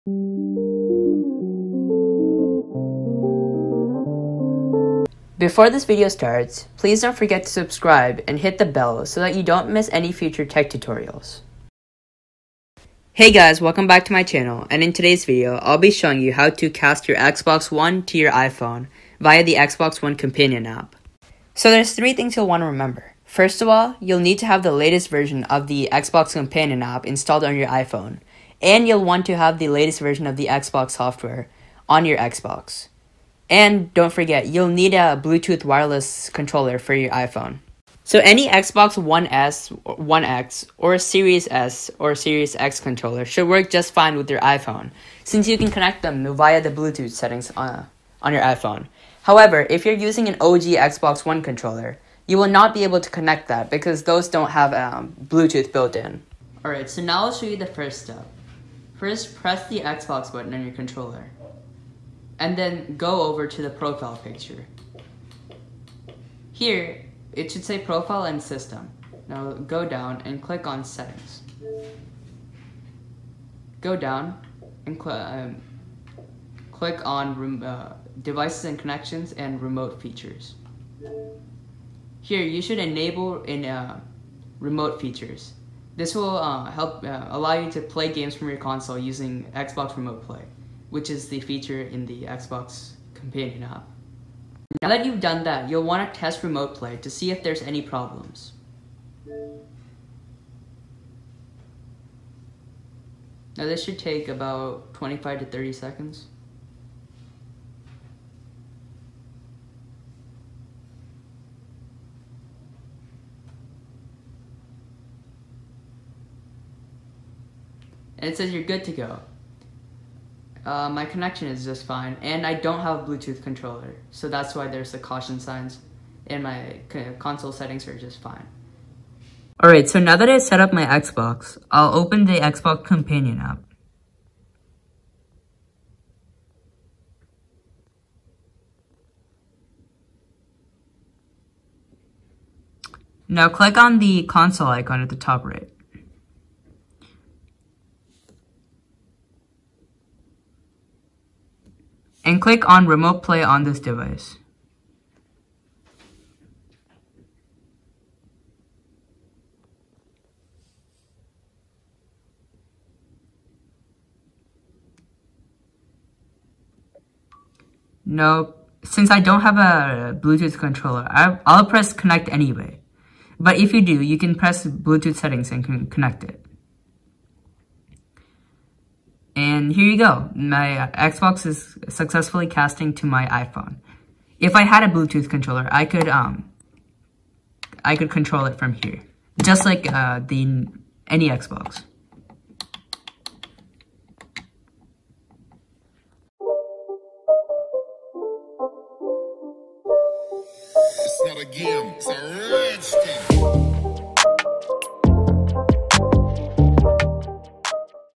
Cast Xbox One to iPhone via Xbox Companion App
(Amazon Link)

(Amazon Link)

In this video, I'll be showing you how to cast your Xbox One to an iPhone via the Xbox Companion App on iPhone. If you found this video informative, please leave a like and subscribe:) If you have any questions, please leave them in the comments section below and I'll try to answer ASAP.